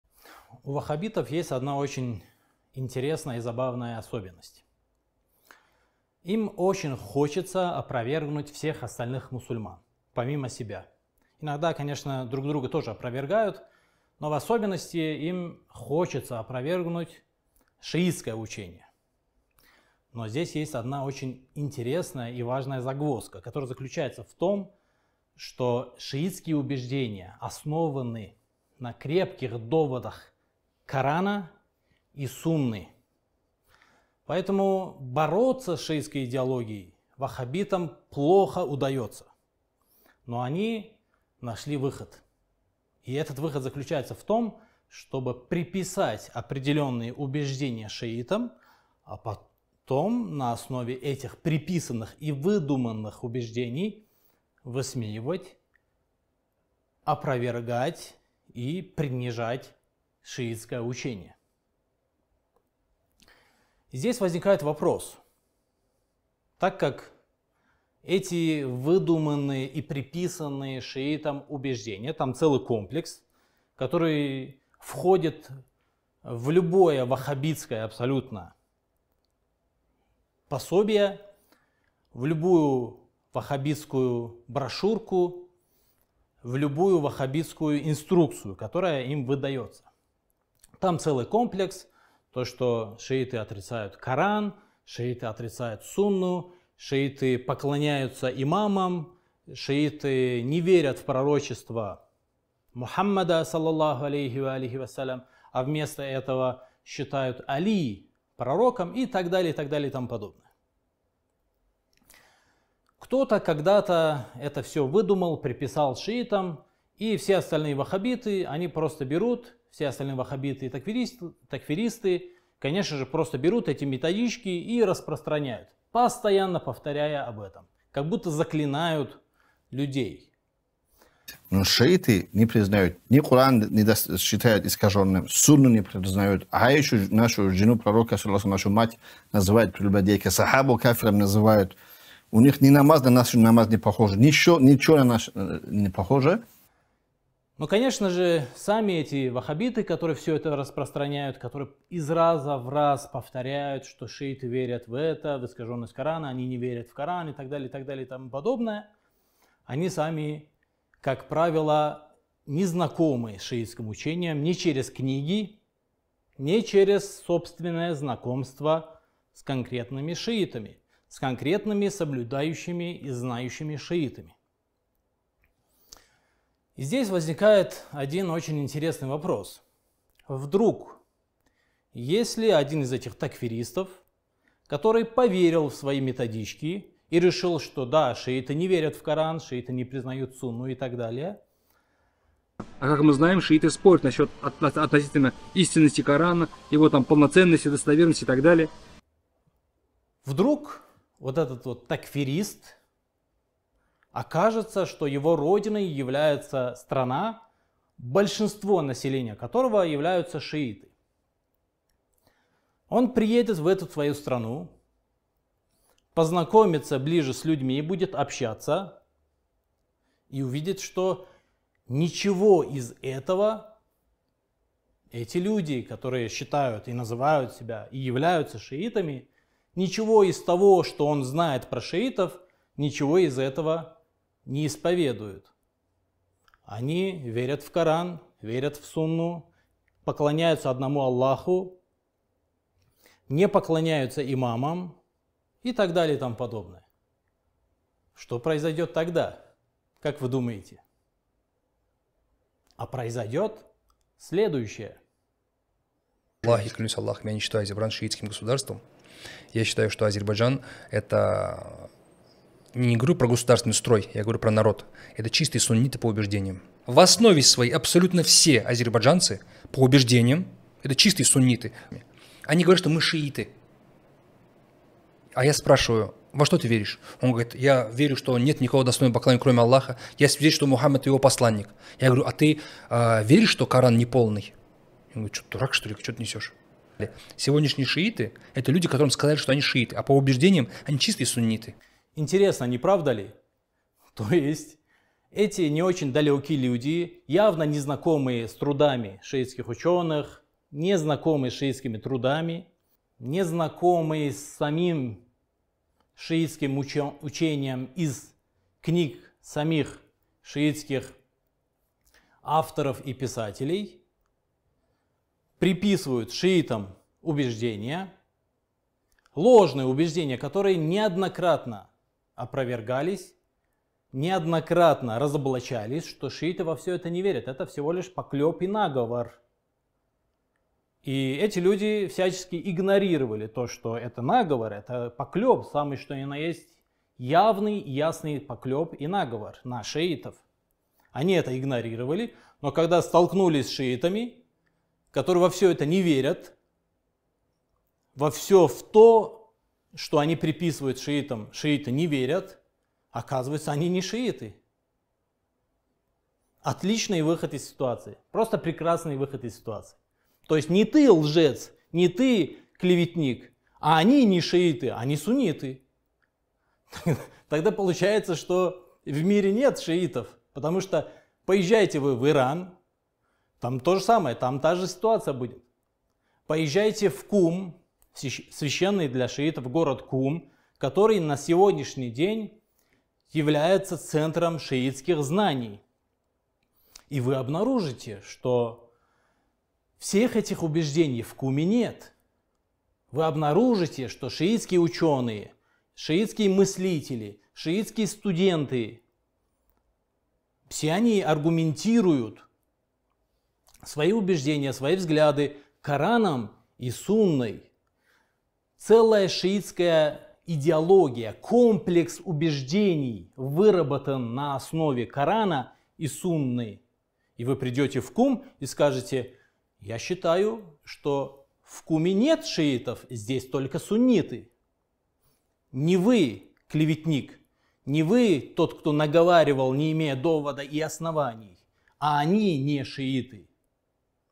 Такфиристы не лгут, просто шиитов не существует!
Ваххабиты не лгут, просто шиитов не существует! Мамед Дневник мусульманина выяснил, что шиизма, как минимум в Азербайджане, не существует, людям внушили, что они шииты, но они по убеждениям сунниты.